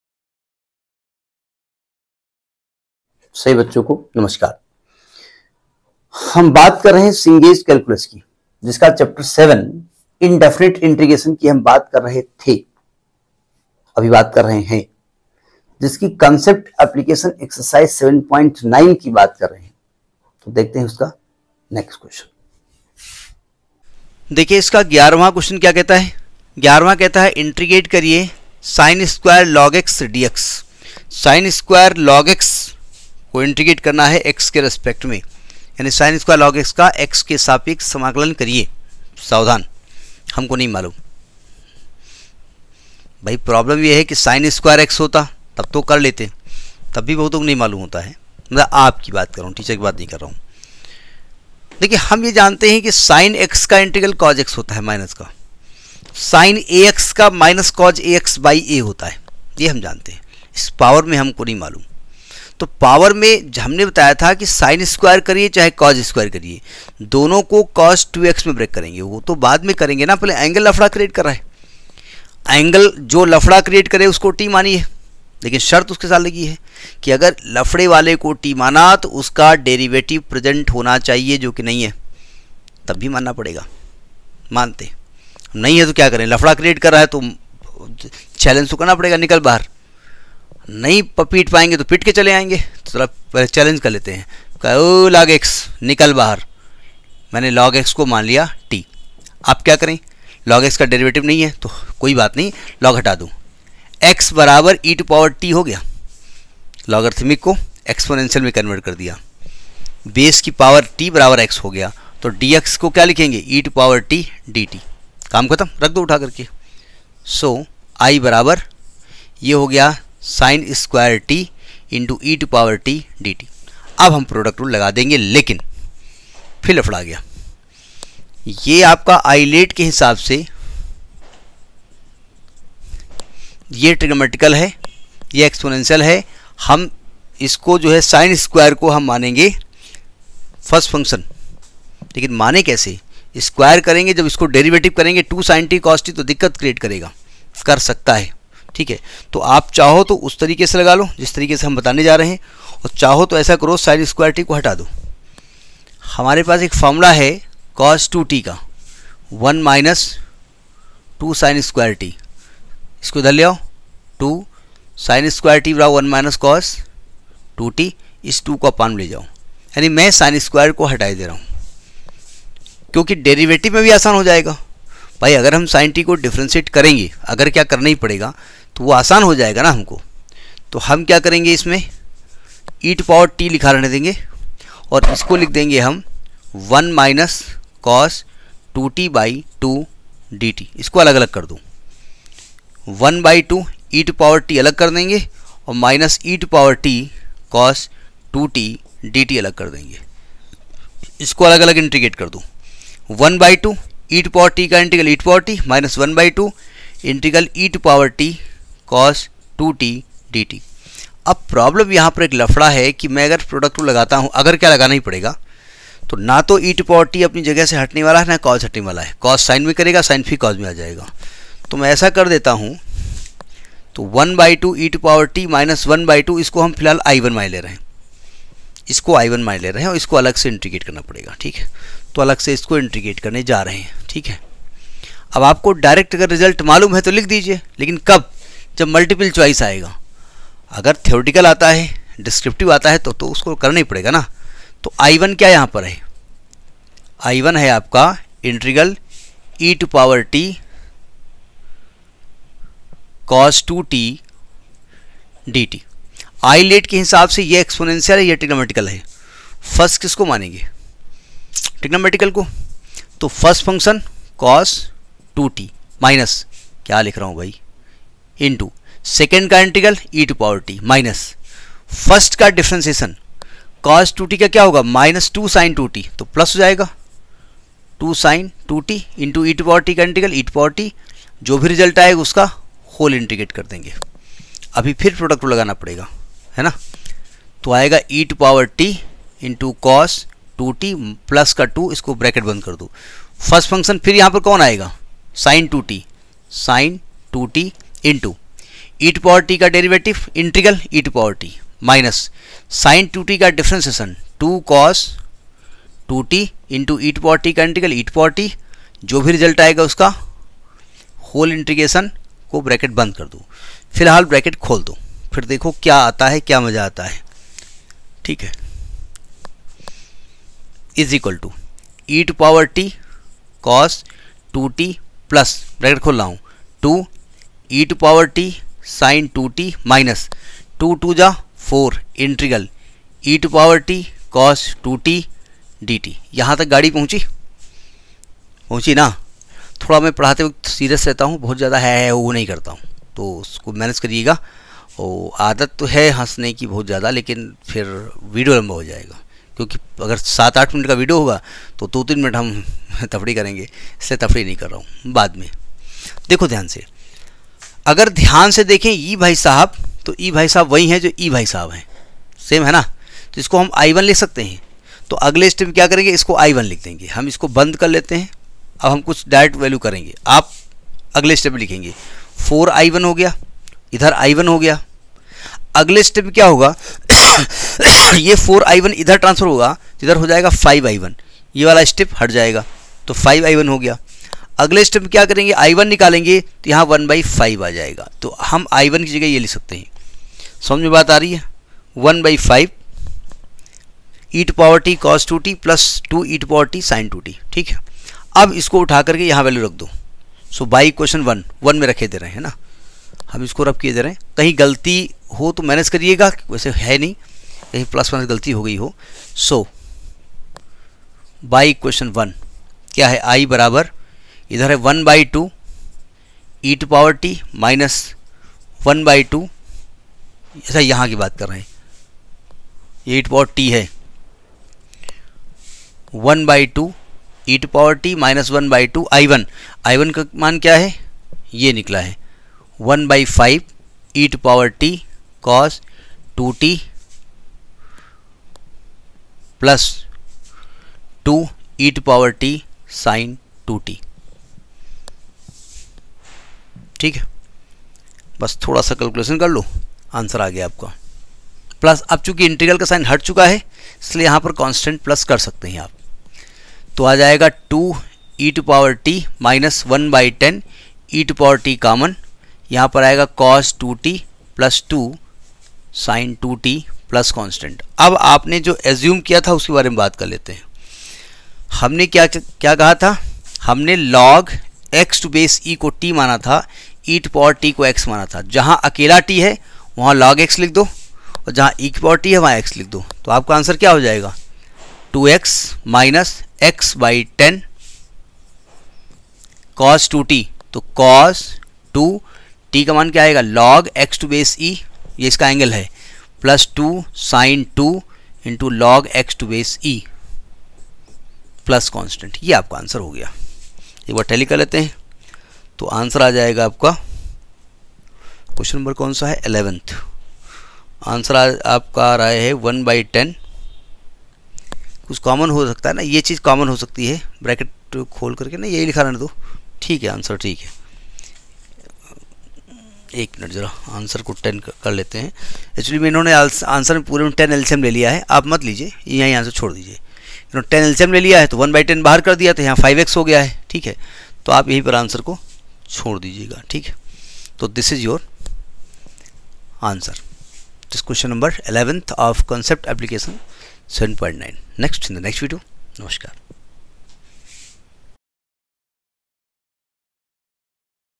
Cengage Calculus Sol.,Chap.(7),Indefinite Integration “Concept Application Ex. 7.9 ( Que.# 11 ).JEE
Cengage Calculus Sol.,Chap.(7),Indefinite Integration “Concept Application Ex. 7.9 ( Que.# 11 ).@AtmaAcademy

In this video I have explained the Indefinite Integral. I have given the complete solution of concept Application Exercise 7.9, Question No.  # 11

Cengage Calculus Sol.,Chap.(7),Indefinite Integral, “Concept Application Ex. 7.9 ( Que. # 11 ),JEE MATHS